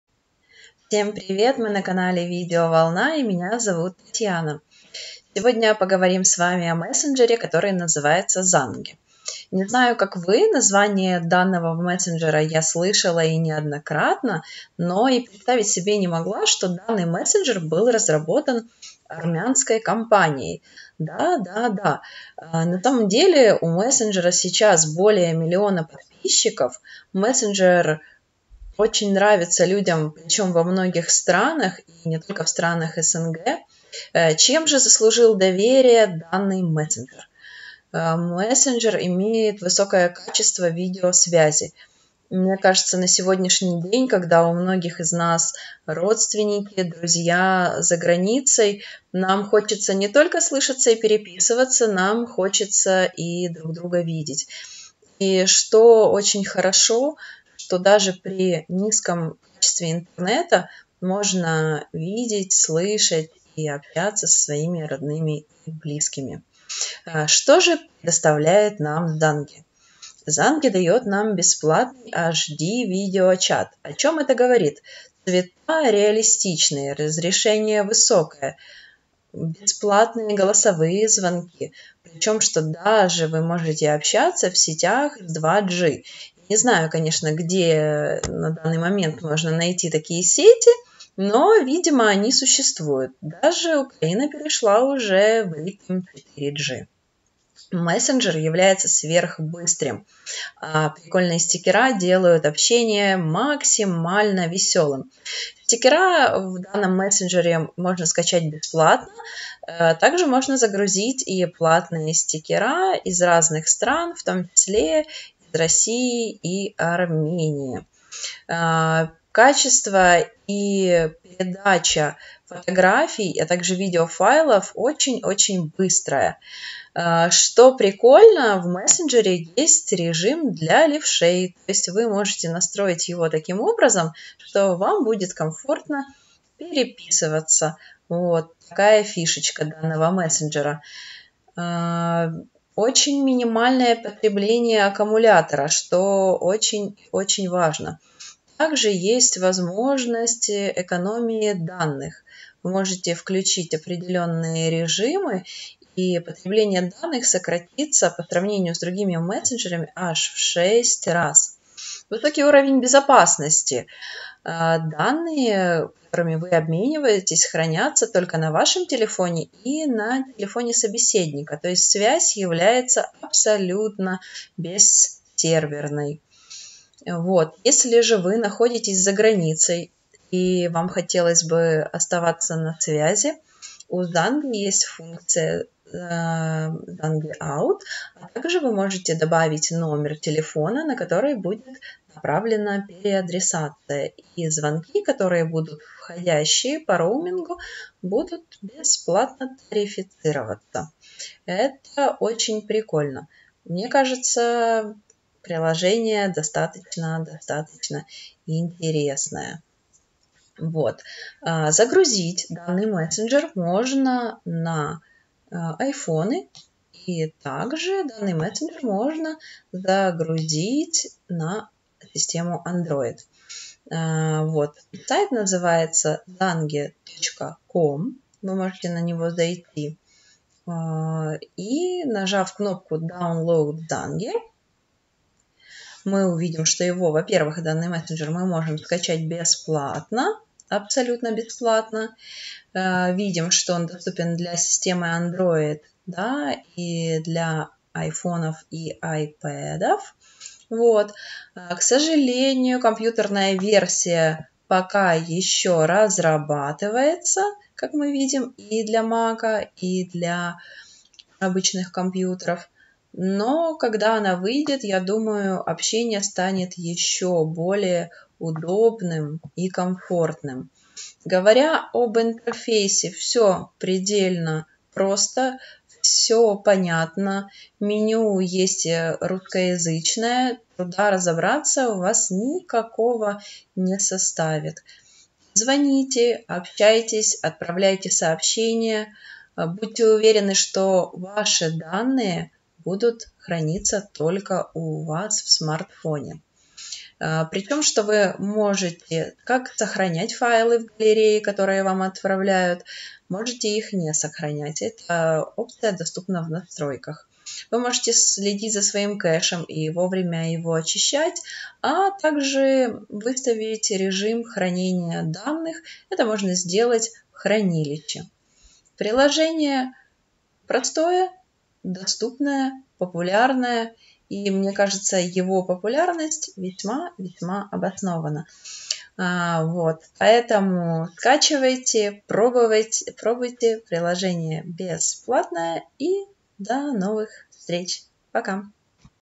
Мессенджер Zangi - “Позвони”
Армянский мессенджер Zangi насчитывает 1 миллион пользователей и считается альтернативой Skype, Viber и WhatsApp. В качестве корпоративного средства связи мессенджер используют компании в 12 странах рынка. Это свидетельствует о том, что Zangi признан на международном рынке и востребован. Качество видео- и аудиосвязи значительно лучше, чем в аналогичных приложениях. Преимуществом мессенджера является то, что он совсем не требовательный к качеству связи. При слабом сигнале все также доступны к отправке текстовые сообщения, аудио- и видеозвонки.